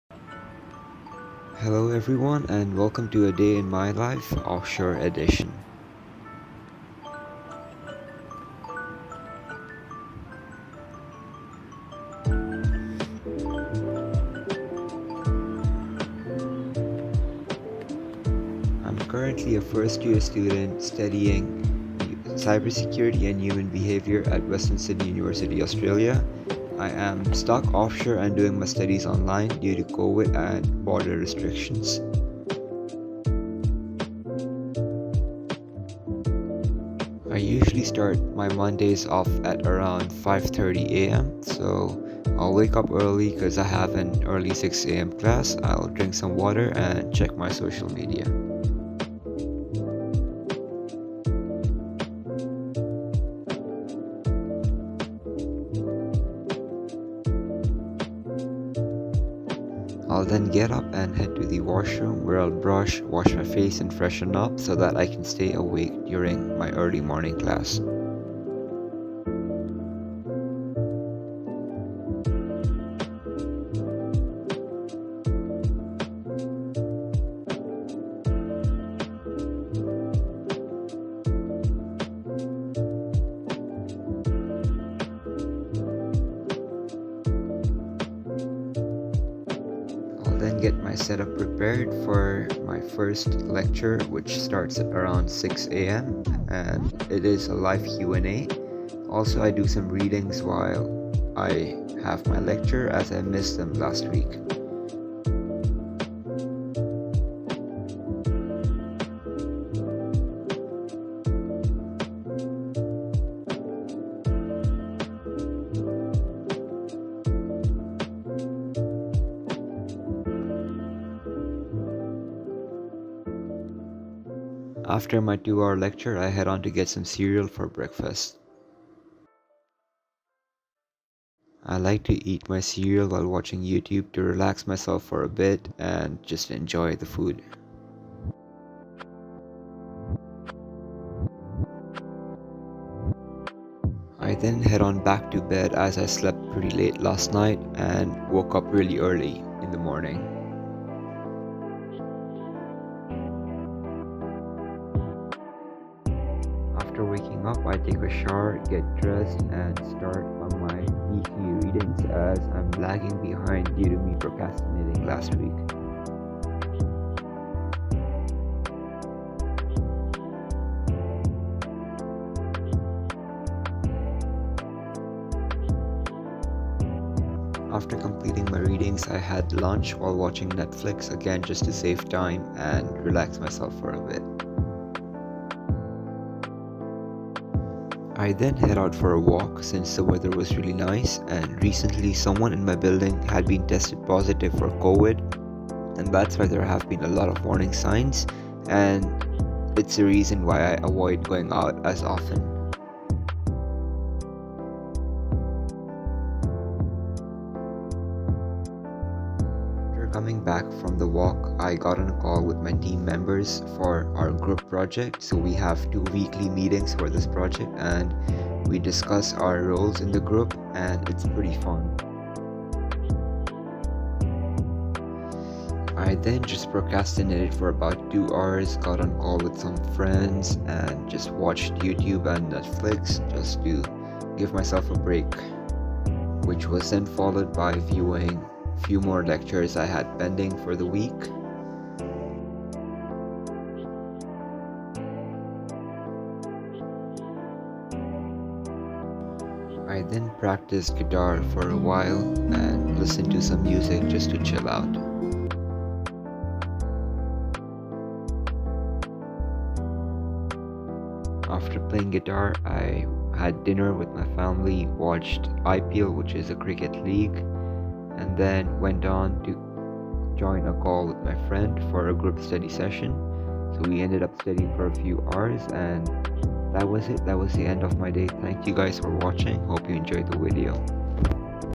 Day In the Life of A Cyber Security Student at Western Sydney University(Offshore Edition)
Hello Everyone,
My name is Kaustav and I am a First-year Cyber Security student at Western Sydney University currently studying online due to border restrictions. This is how a typical day in my life would go let me know if you have suggestions for future videos in the comments.
Cheers!